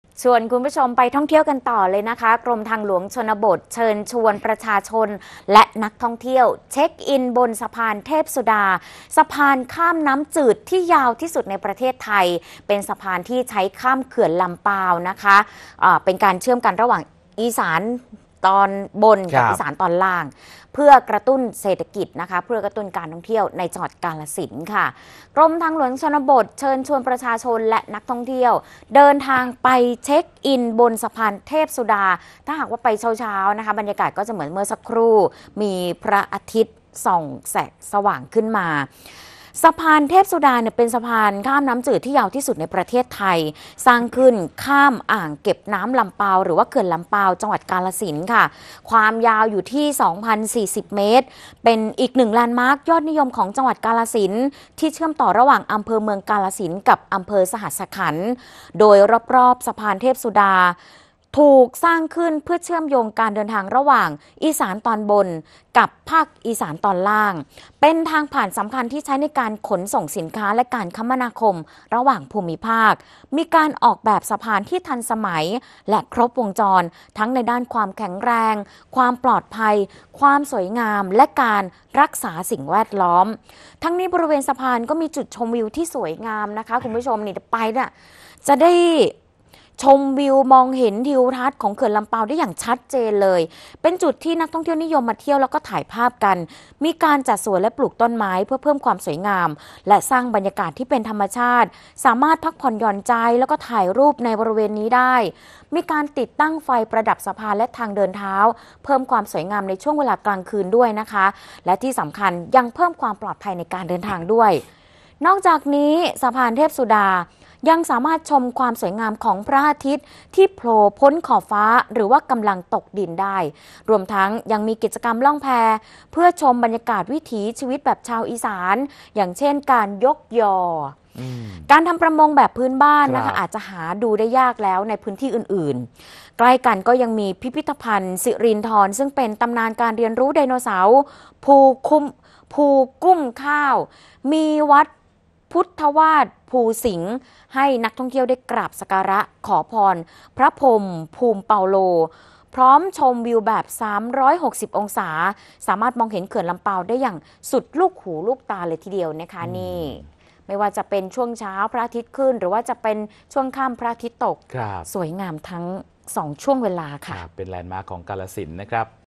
ทช. ชวนเช็คอิน “สะพานเทพสุดา” สะพานข้ามน้ำจืดยาวที่สุดในไทย ข่าวเช้า วันที่ 23 ตุลาคม 2567
กรมทางหลวงชนบท เชิญชวนประชาชนและนักท่องเที่ยว เช็คอินบนสะพานเทพสุดา สะพานข้ามน้ำจืดที่ยาวที่สุดในประเทศไทย ข้ามเขื่อนลำปาว เพื่อกระตุ้นเศรษฐกิจการท่องเที่ยวจังหวัดกาฬสินธุ์